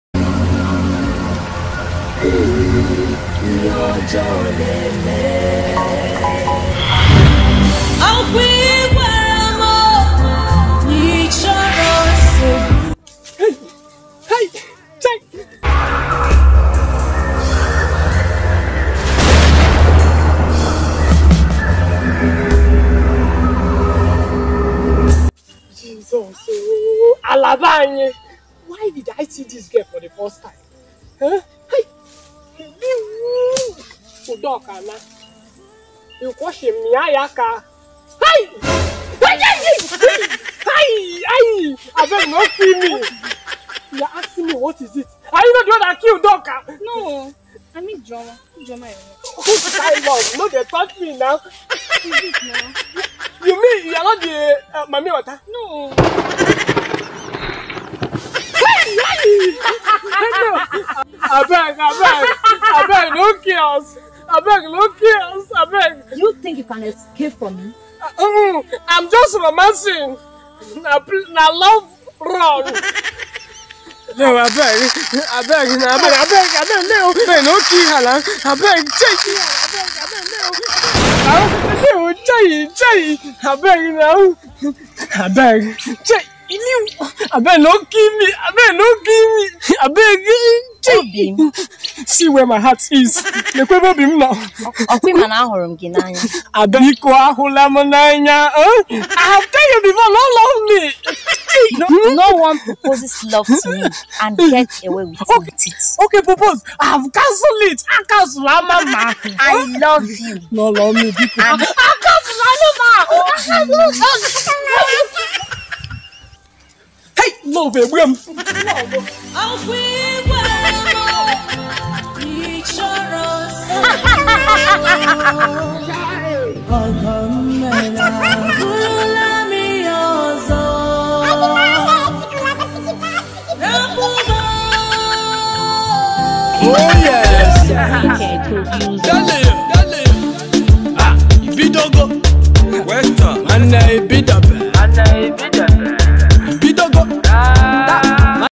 mamy water love episode 2
Durable toast mamy water , she kill Durable girl friend and still insist to marry Durable pleas watch and see what happens to Durable brodashahggi sabinis # sir one # joke $ comedy